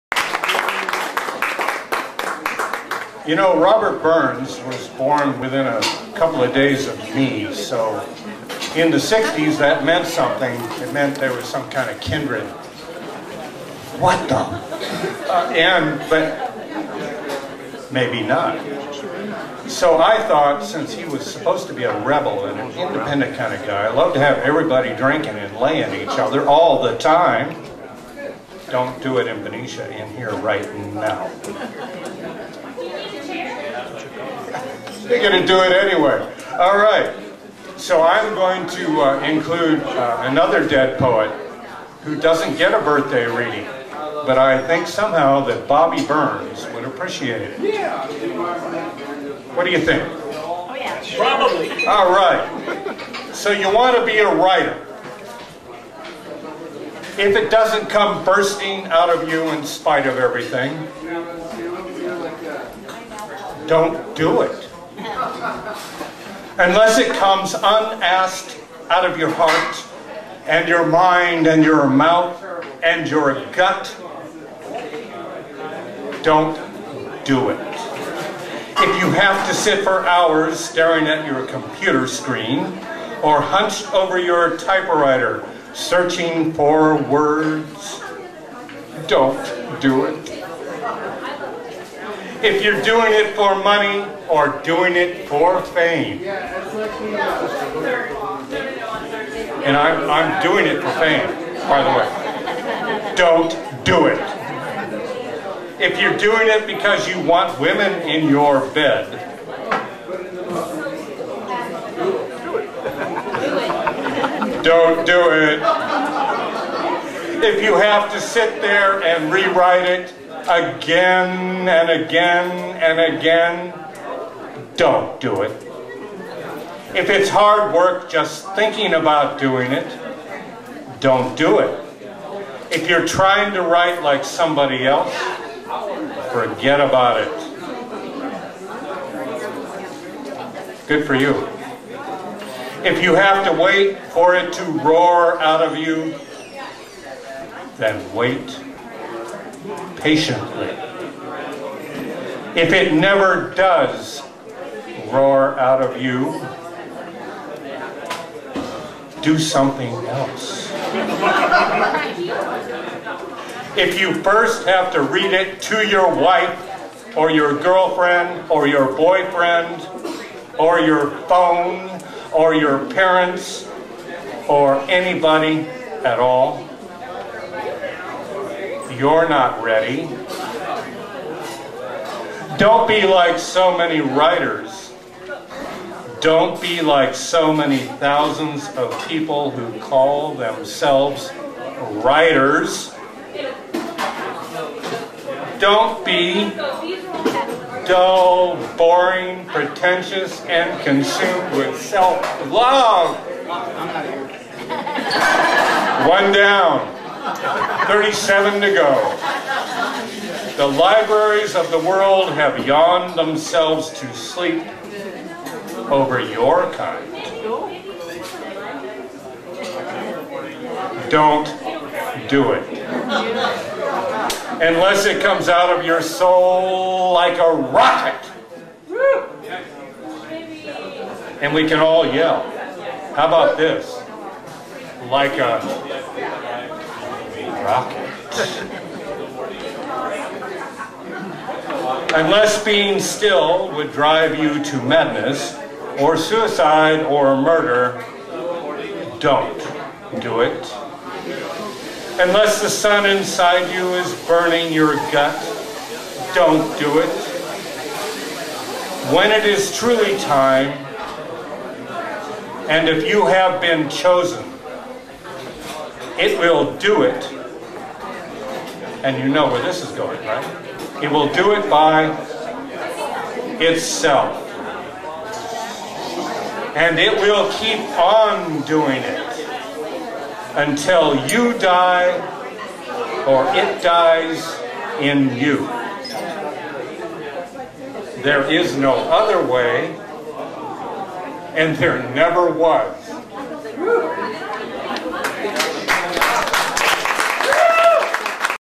"So You Want To Be a Writer?" by Charles Bukowski, read by Tom Stanton
Read live at 'The Rellik Tavern' by Tom Stanton on January 25th, 2016.

"So You Want To Be a Writer?"
from a poem by Charles Bukowski, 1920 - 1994

"if it doesn’t come bursting out of you
in spite of everything,